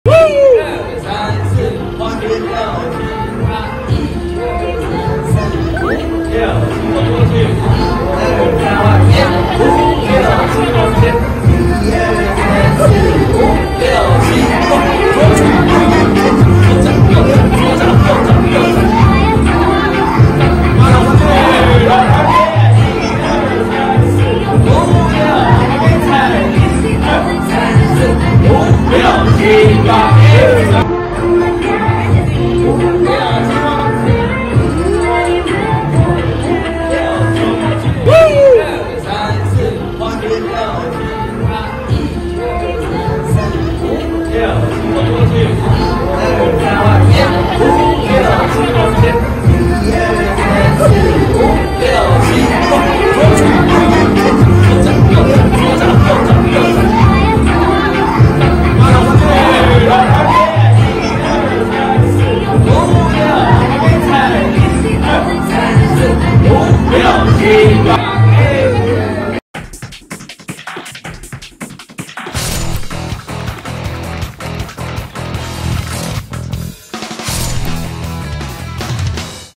比韓國女團還辣！新手爸舞力全開 背小孩大跳韓舞OMG｜三立娛樂星聞
來源:FB/ 媽咪寶貝情報站 授權